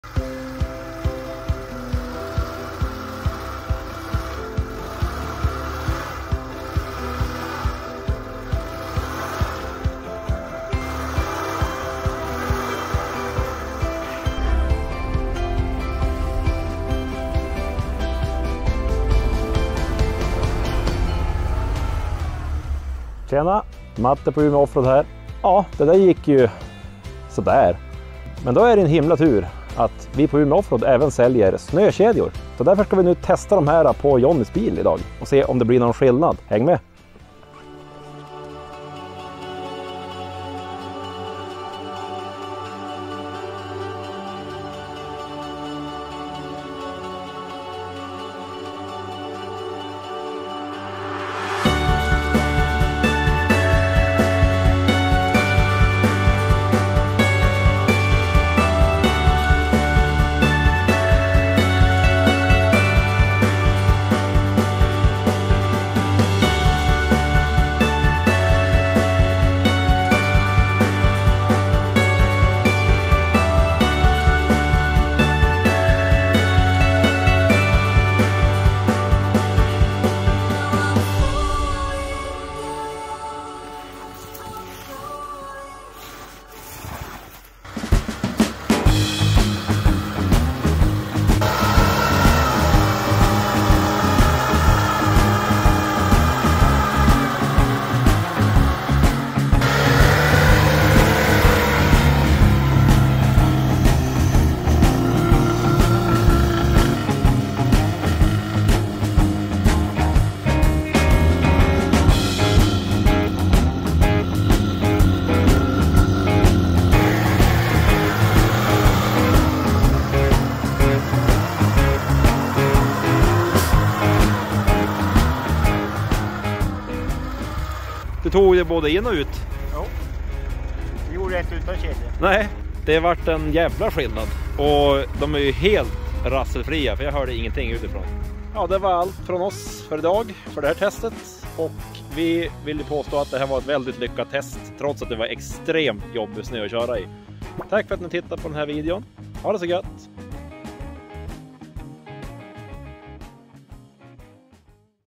MONTERING O TEST AV SNÖKEDJOR (FULLVERSION)
Monteringsanvisningar och extremtest av våra standardkedjor ifrån Umeå Offroad & Import AB

Länk till våra snökedjor!